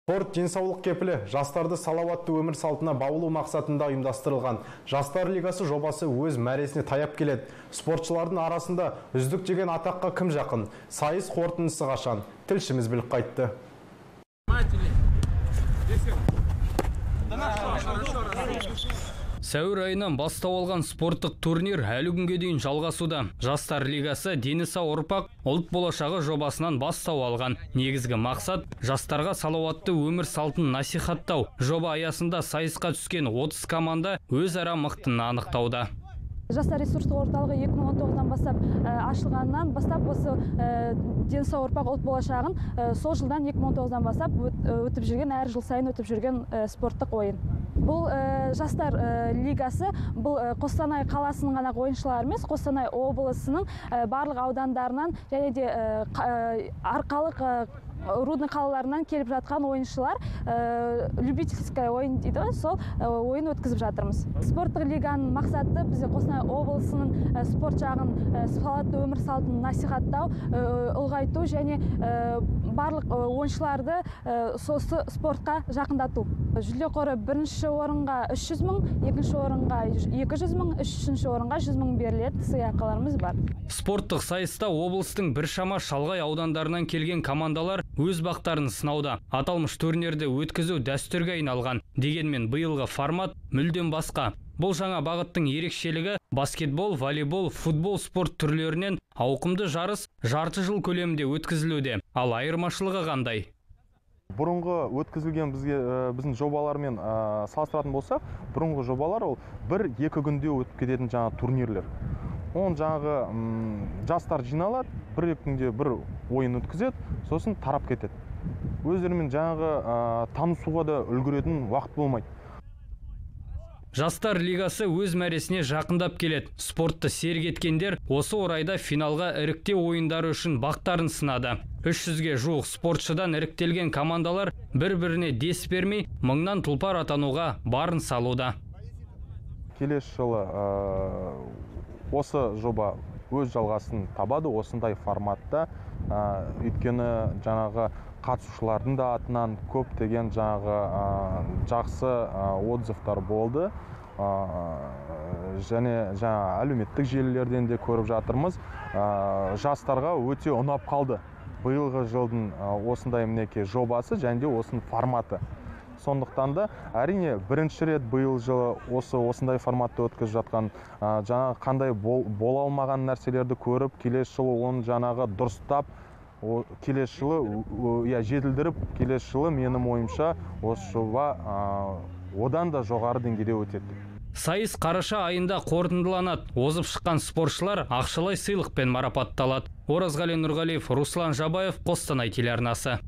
«ЖАСТАР ЛИГАСЫ» мәреге жақындады
Спорт – денсаулық кепілі. Жастарды салауатты өмір салтына баулу мақсатында ұйымдастырылған «Жастар лигасы» жобасы өз мәресіне таяп келеді. Спортшылардың арасында «Үздік» деген атаққа кім жақын? Сайыс қорытындысы қашан?